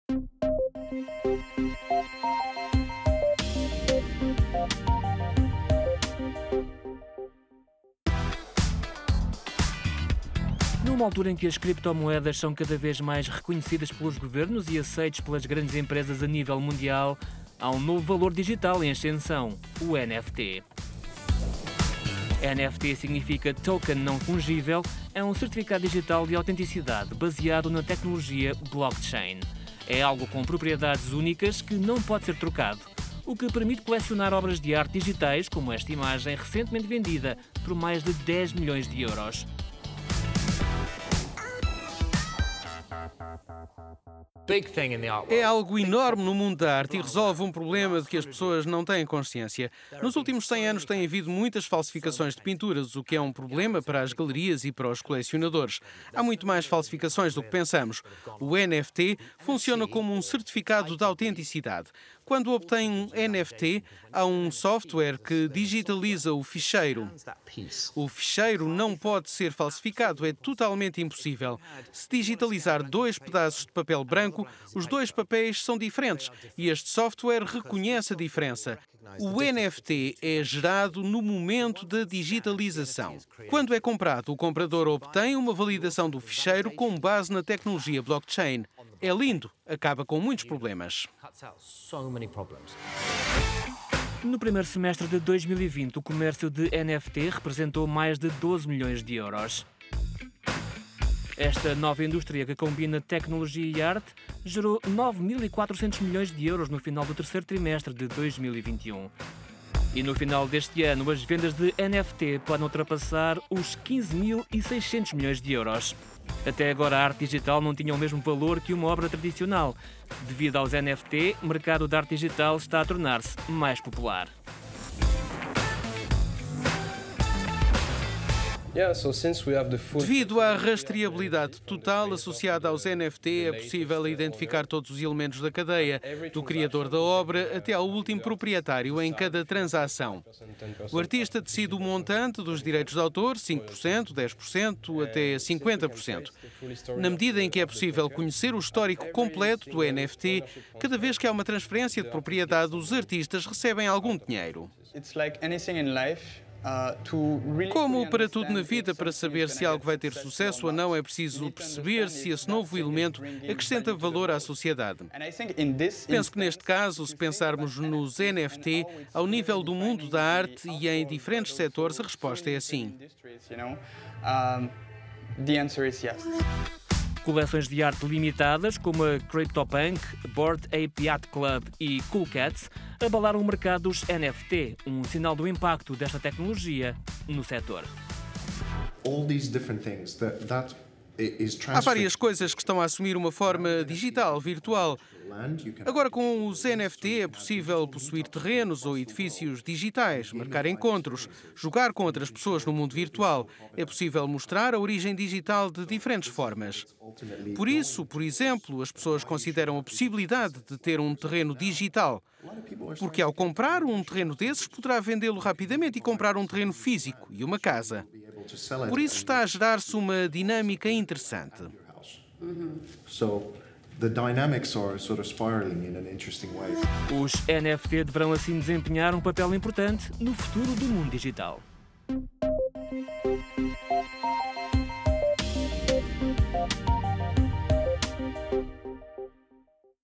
O NFT está a revolucionar o mundo da arte. O que é o NFT?
Graças aos NFT, uma imagem digital foi vendida por dez milhões de euros.